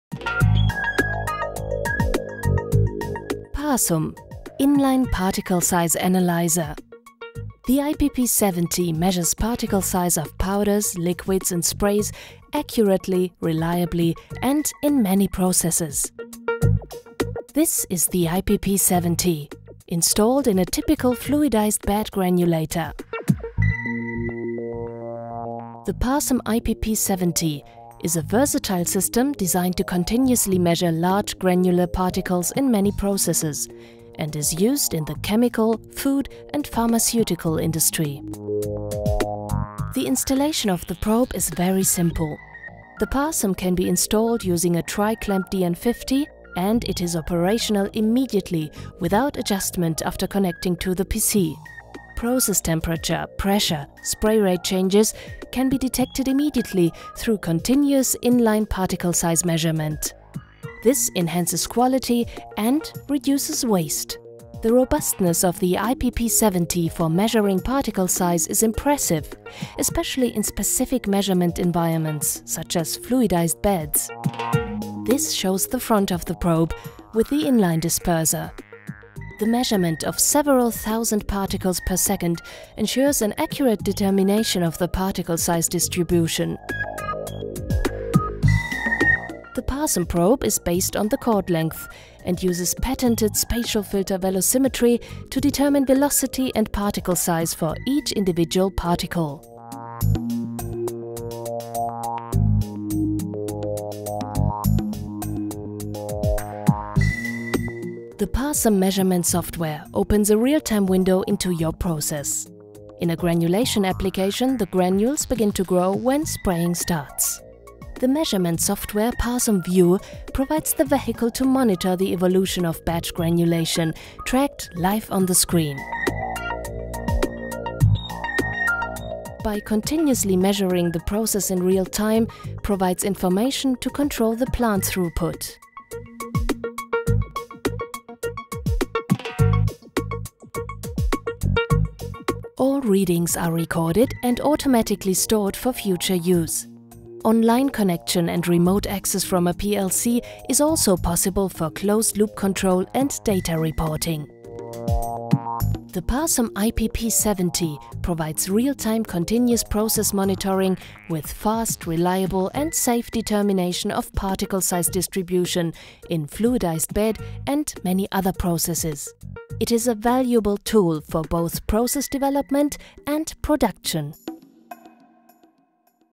Parsum in-line particle size analyser using spatial filter velocimetry
Measure particle size and distribution with the Parsum probe IPP70 in a granulator during the process, using spatial filter velocimetry. Therefore the size distribution of the particles is tracked through the entire batch. Whether coating or agglomeration, grinding or spray-drying, the process remains under control, in real time!